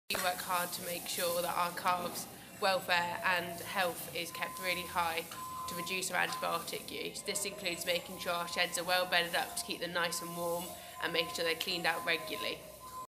Charlotte, a calf rearer from Dorset, talks about what she does to keep her calves healthy...
To support the launch of the the second RUMA Targets Task Force Report in 2020, farmers and vets across the UK sent in clips of film explaining how they had managed reductions in antibiotic use. They also talked about what they saw happening around them in terms of changes in antibiotic stewardship across the whole UK farming industry.

Here, Charlotte, a calf rearer from Dorset, talks about the comfort and hygiene focus in her rearing, and how that helps reduce disease and antibiotic use.